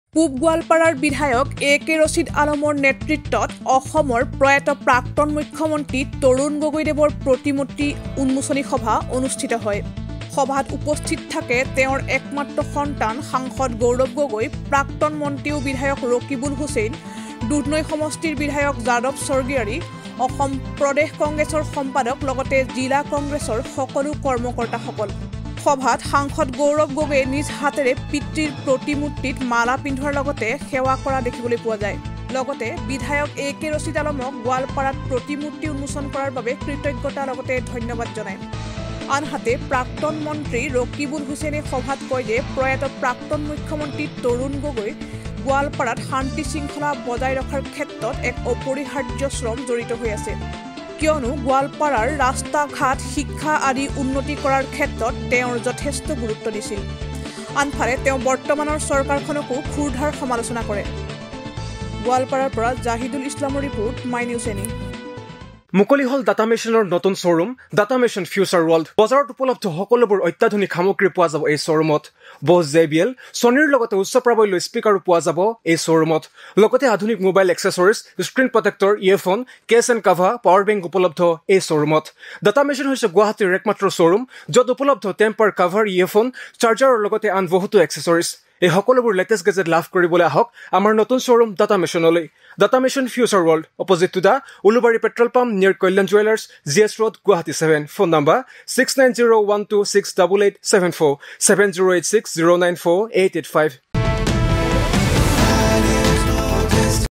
প্ৰয়াত প্ৰাক্তন মুখ্যমন্ত্ৰী তৰুণ গগৈ দেৱৰ প্ৰতিমূৰ্তি উন্মোচন
My News NE:
পূৱ গোৱালপাৰাৰ বিধায়ক এ কে ৰচিদ আলমৰ নেতৃত্বত অনুষ্ঠিত উন্মোচনী সভা
সভাত উপস্থিত প্ৰয়াত মুখ্যমন্ত্ৰী গৰাকীৰ পুত্ৰ সাংসদ গৌৰৱ গগৈ
প্ৰতিমূৰ্তিত মালা অৰ্পণ গৌৰৱ গগৈৰ
সভাত উপস্থিত বিধায়ক ৰকিবুল হুছেইন
About Channel: